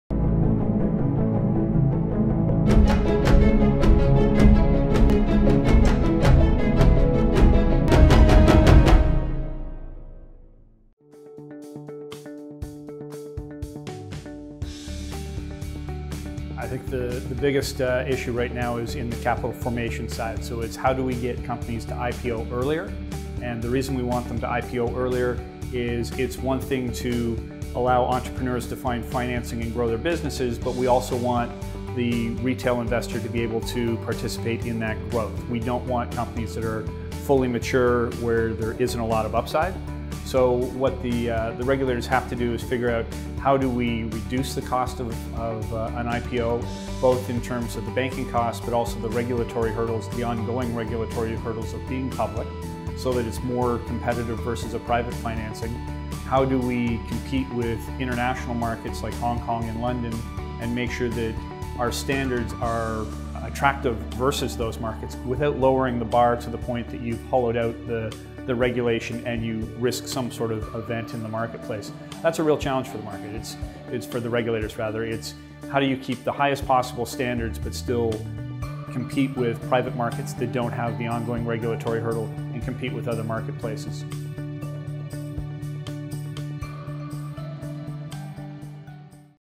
Doug Clark | Equity Market Structure
Doug Clark | ITG Managing Director on Equity Market Structure

What is the greatest regulatory item on the equity market's radar?